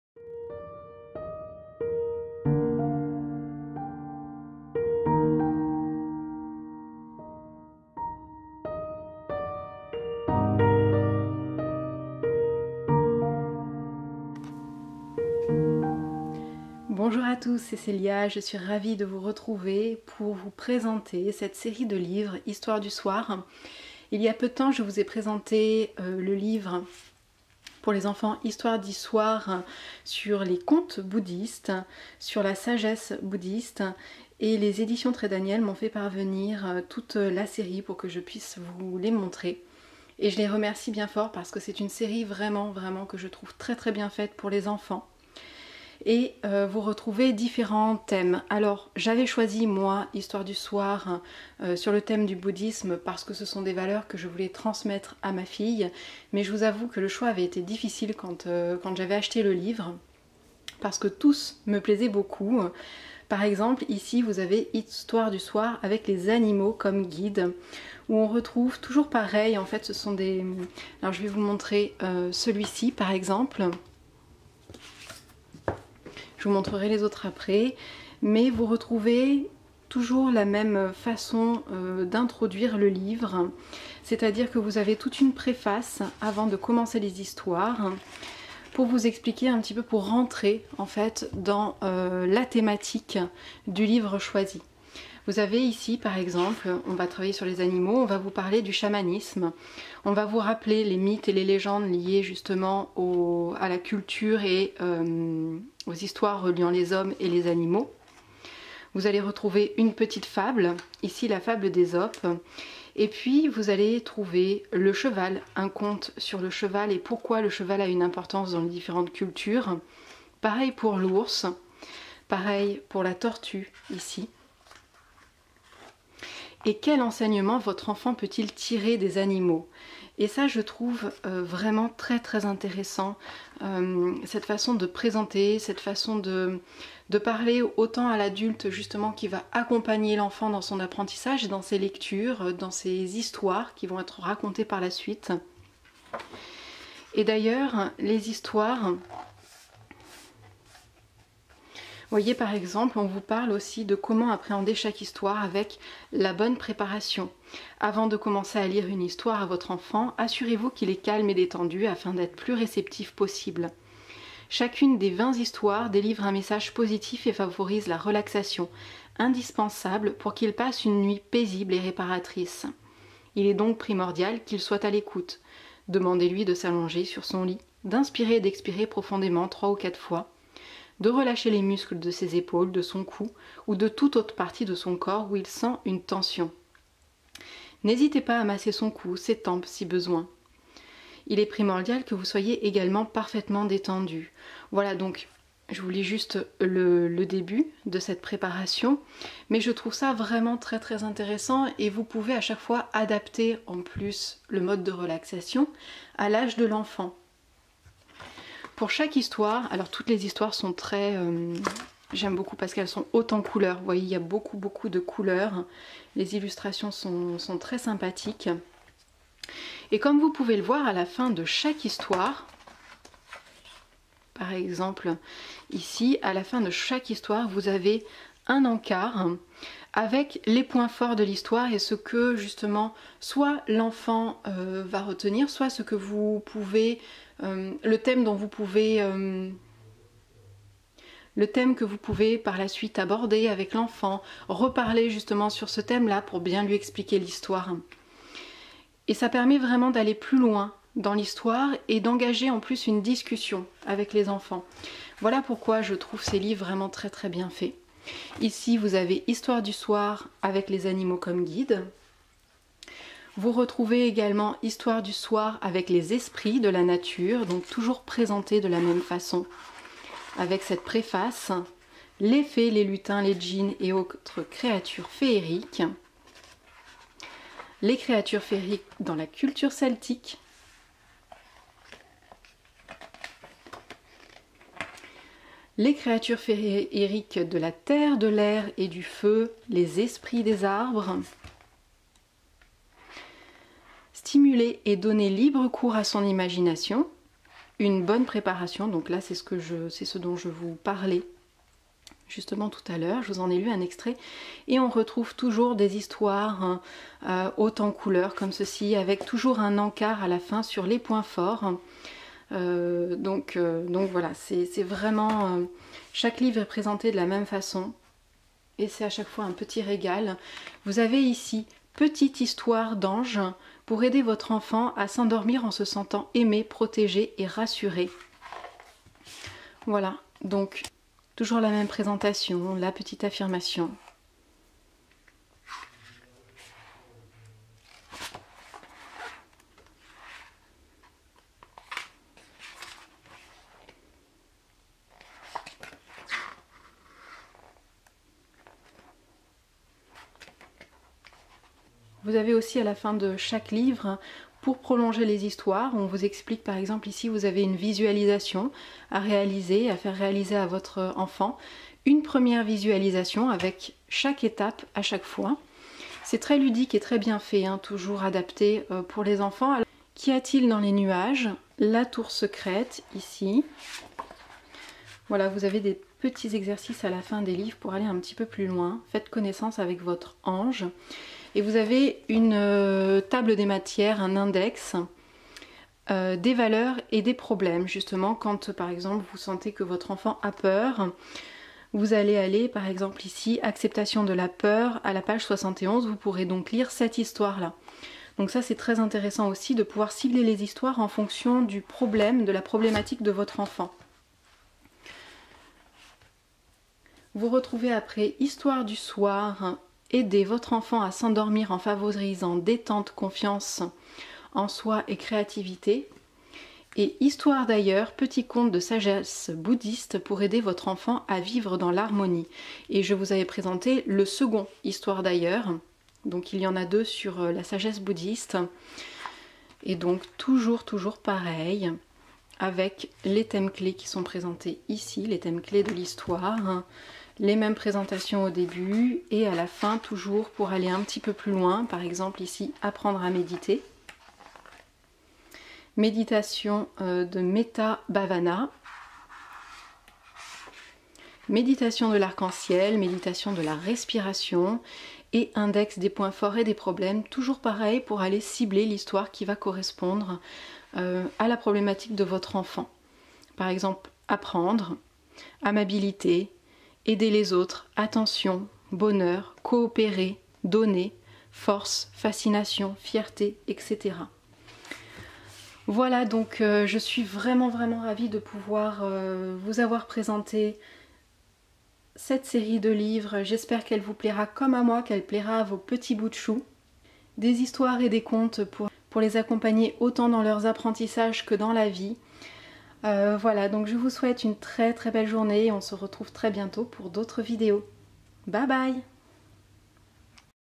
La collection Histoires du soir, une merveille pour nos petit e s
Où trouver :
Histoire du soir avec les animaux comme guide
Petites histoires d'anges
Histoires du Soir : Petites leçons de vie bouddhistes
Histoires d'ailleurs : Petits contes de sagesse bouddhiste :
Histoires du soir avec les esprits de la nature
Histoires du soir : Aider votre enfant à s'endormir en favorisant détente, confiance en soi et créativité
Nouvelles histoires du soir : Aider votre enfant à s'endormir

♛Pour commander l'Oracle des Reflets / Le Tanis Lenormand / Les Lili White et Lili Black Tarot : ♛